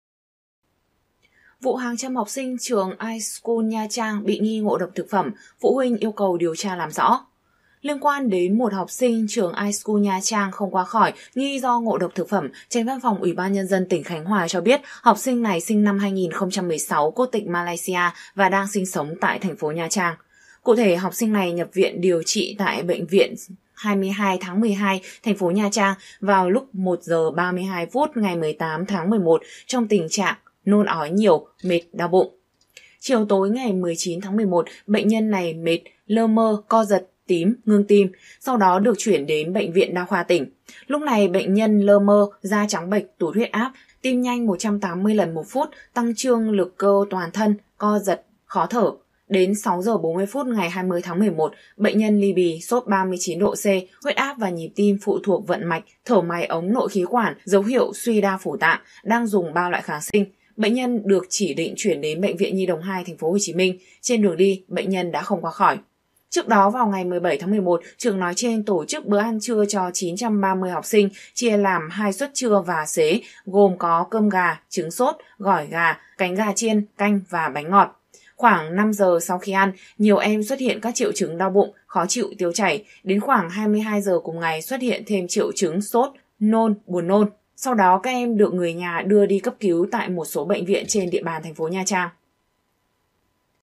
Diễn biến vụ học sinh iSchool Nha Trang ngộ độc: Phía phụ huynh nói gì? | TV24h
Phụ huynh yêu cầu tìm chuyên gia, bác sĩ giỏi đến Nha Trang và có phác đồ điều trị cho học sinh

Các bệnh viện đã tiếp nhận 600 học sinh vào theo dõi, điều trị

Chiều 20/11, Văn phòng UBND tỉnh Khánh Hòa đã có thông cáo báo chí về việc ngộ độc tập thể tại Trường Tiểu học, THCS&THPT iSchool Nha Trang (gọi tắt là iSchool Nha Trang).

Theo đó, ngày 17/11, tại Trường iSchool Nha Trang tổ chức bữa ăn trưa cho 880 học sinh được chia làm 2 suất lúc 10g30 và 11g30; bữa ăn xế lúc 13g30.

Khoảng 5g sau khi ăn (vào lúc 16g30 – 17g ngày 17/11), một số em xuất hiện các triệu chứng đau bụng, khó chịu, tiêu chảy nhiều lần. Đến khoảng 22g ngày 17/11, xuất hiện thêm triệu chứng sốt, buồn nôn, nôn. Đến 22g30 ngày 17/11, các em được người nhà đưa đi nhập viện tại các bệnh viện trong thành phố.

Tính đến 11g ngày 20/11, qua báo cáo nhanh của Sở Y tế, tình hình tiếp nhận, xử trí các ca ngộ độc thực phẩm tại các đơn vị y tế, đã tiếp nhận tổng cộng 600 ca. Trong đó, có 240 ca xử trí ổn định cho về theo dõi và 360 ca nhập viện điều trị nội trú. Đến nay, đã có 93 ca xuất viện, 266 ca đang điều trị và 1 ca tử vong.

Học sinh tử vong tên L.Z.X., sinh năm 2016, ngụ ở xã Vĩnh Hiệp, TP. Nha Trang. Bệnh nhân này nhập viện lúc 1g32 ngày 18/11 tại Bệnh viện 22/12 với triệu chứng nôn ói, đau bụng, mệt. Đến chiều tối 19/11, bệnh nhân co giật, tím, ngưng tim nên được hồi sức có tim, chuyển Bệnh viện đa khoa tỉnh Khánh Hòa. Đến sáng 20/11, em này sốt cao, huyết áp và nhịp tim phụ thuộc vận mạch, thở máy ống nội khí quản, dấu hiệu suy đa phủ tạng nên chỉ định chuyển vào TPHCM. Sau đó, bệnh nhân tử vong trên đường chuyển viện.

Sau khi vụ việc xảy ra, Chi cục An toàn Vệ sinh thực phẩm - Sở Y tế đã tổ chức Đoàn điều tra ngộ độc thực phẩm kết hợp với địa phương, Ban Giám hiệu trường học kiểm tra thực tế việc chấp hành các quy định pháp luật về vệ sinh an toàn thực phẩm đối với bếp ăn tập thể tại trường iSchool. Kết quả kiểm tra: "Cơ sở chấp hành các quy định pháp luật về vệ sinh an toàn thực phẩm, chưa phát hiện vi phạm về an toàn thực phẩm''.

Đoàn điều tra đã lập biên bản lấy mẫu và niêm phong toàn bộ mẫu thức ăn được lưu tại cơ sở đối với thực đơn bữa ăn trưa, bữa ăn xế ngày 17/11 gửi về Viện Pasteur Nha Trang để xét nghiệm tìm nguyên nhân gây ngộ độc thực phẩm. Hiện, đang chờ Viện Pasteur Nha Trang nuôi cấy và phân lập định danh vi khuẩn, xác định nguyên nhân độc tố. Dự kiến, đến thứ Tư (ngày 23/11) mới có kết quả từ phía Viện Pasteur Nha Trang.

Tại buổi đối thoại, nhiều phụ huynh cũng đưa ra các vấn đề liên quan đến vụ việc này. Trong đó, phụ huynh yêu cầu nhà trường không chỉ xem xét về vấn đề thức ăn mà phải kiểm tra cả nguồn nước uống để tìm nguyên nhân ngộ độc; đề nghị nhà trường, Tập đoàn Nguyễn Hoàng tìm chuyên gia, bác sĩ đầu ngành tới Nha Trang để hỗ trợ điều trị cho học sinh; có phác đồ điều trị cụ thể… Đồng thời, nhà trường phải làm rõ nguyên nhân ngộ độc vì 4 ngày rồi chưa có kết quả. Ngoài ra, phụ huynh yêu cầu phải tìm những nhà cung cấp thức ăn, nguồn cung thực phẩm, chuyên gia dinh dưỡng và bữa ăn phải theo độ tuổi của học sinh…